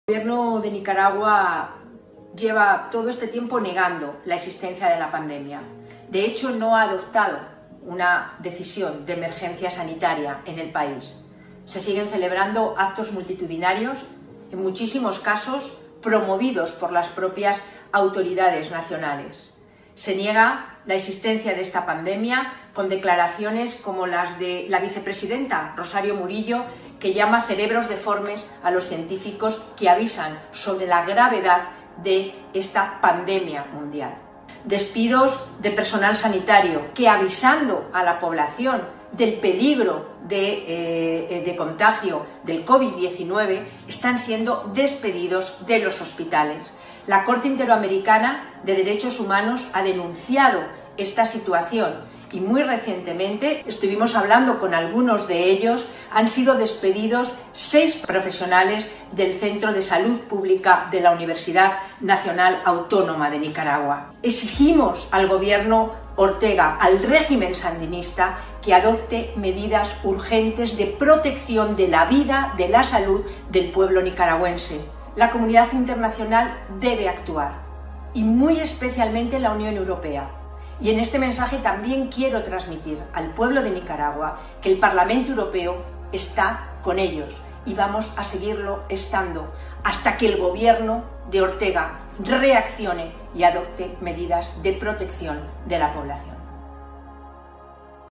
Eurodiputada pide al Gobierno de Nicaragua tome medidas de protección a la población ante covid-19
Eurodiputada Soraya Rodríguez “Pedimos al Gobierno de Nicaragua la adopción de medidas de protección de la población frente al Covid -19 y que cesen los despidos y hostigamiento al personal sanitario que está alertando a la población sobre las graves consecuencias de la pandemia”